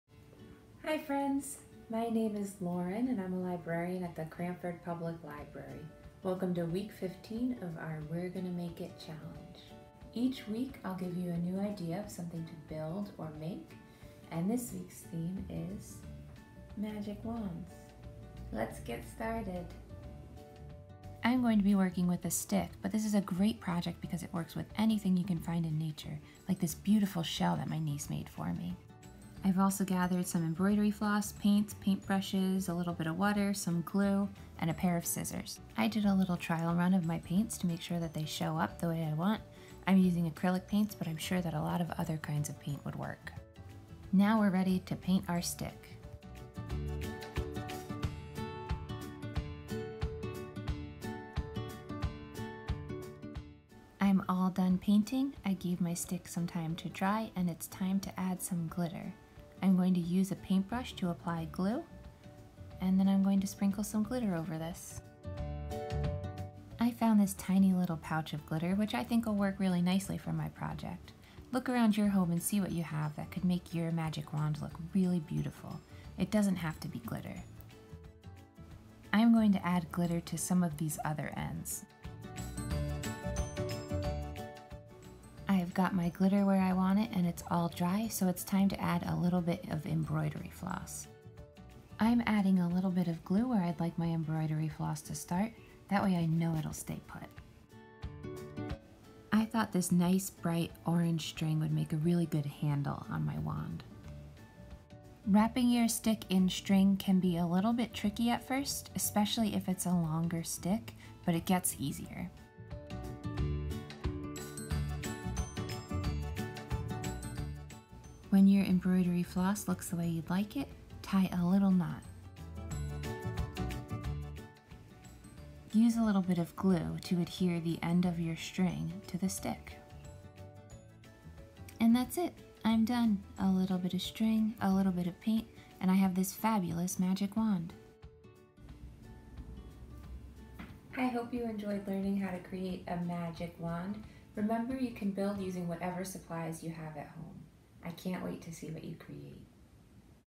Episode 15: "Magic" Wand
This week we're turning twigs and sticks into magic wands!